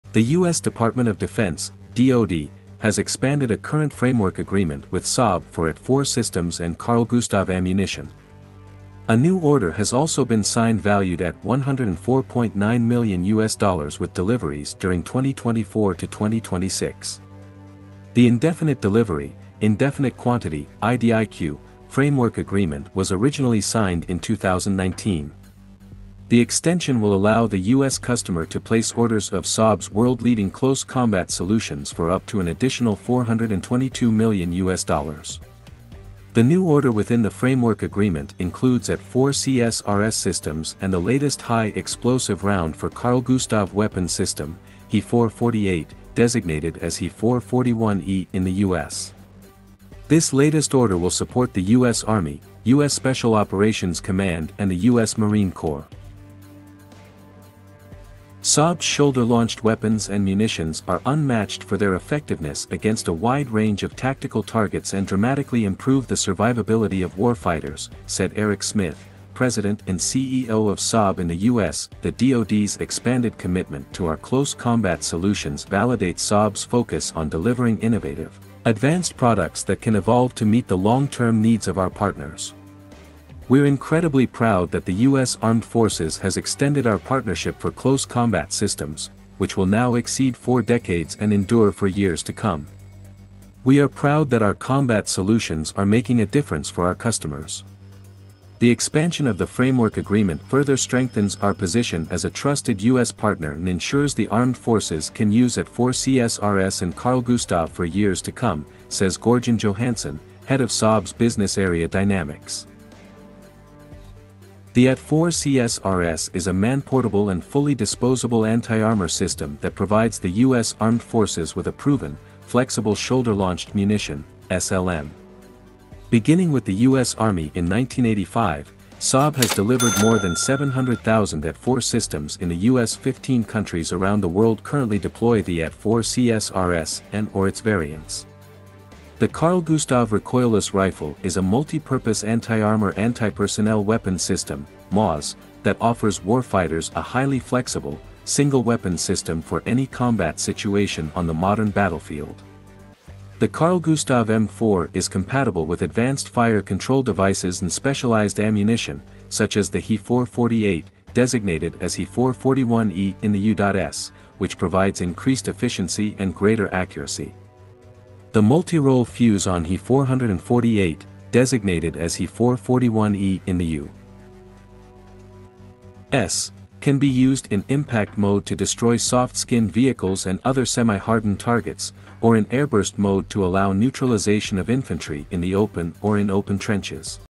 Saab to deliver additional AT4 and Carl Gustaf anti tank anti personnel ammunition to USSOCOM and US
The U.S. Department of Defense (DoD) has expanded a current framework agreement with Saab for AT4 systems and Carl-Gustaf ammunition. A new order has also been signed valued at USD 104.9 million with deliveries during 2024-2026.